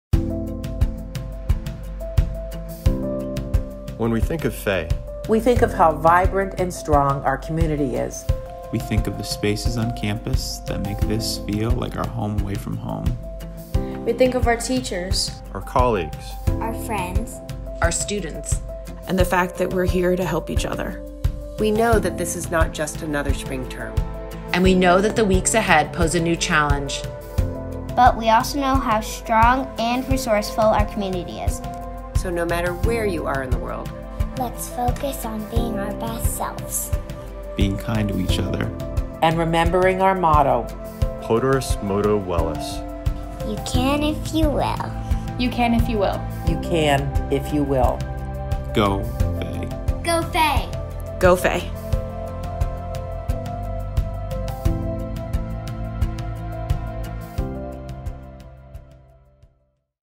You Can If You Will
A few thoughts from our community members as Fay begins a spring term unlike any other.